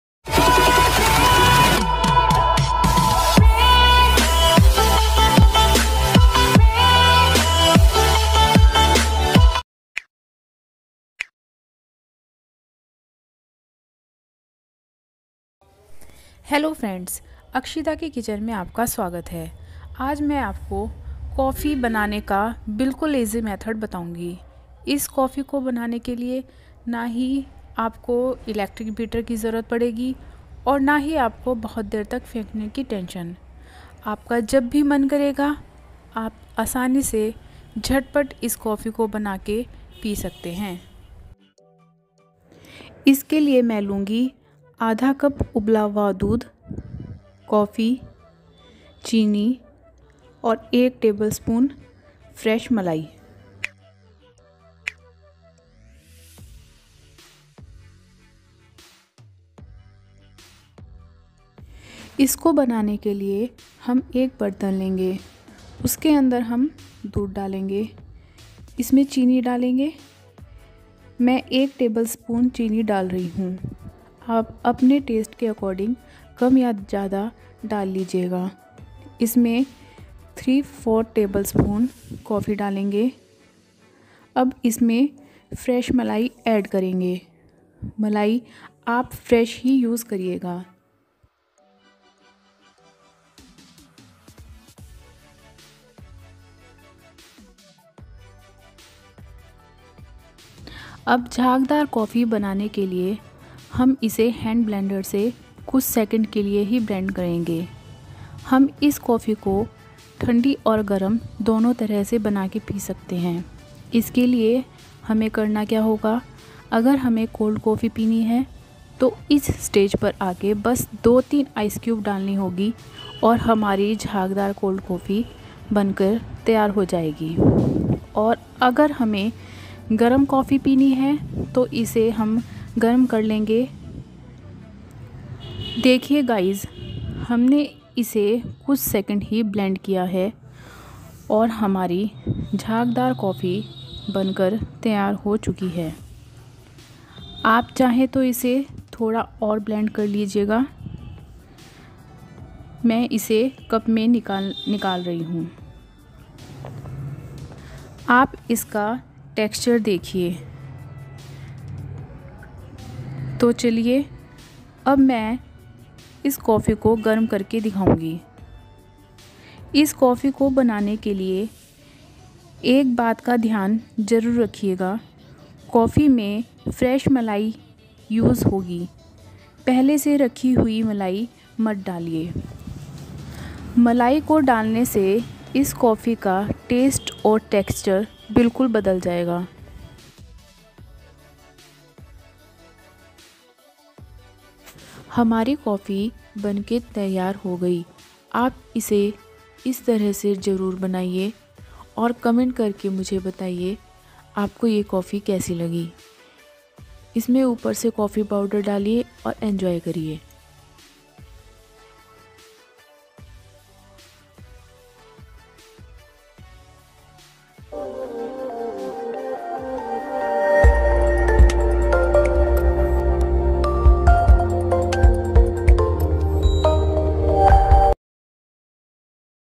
How to make instant coffee at home | घर पर बनाएं रेस्टोरेंट जैसी टेस्टी कॉफ़ी
Today, Akshita ka Rasoda presents you an easy and tasty recipe for instant coffee which can be made with very less ingredients.

Thanks.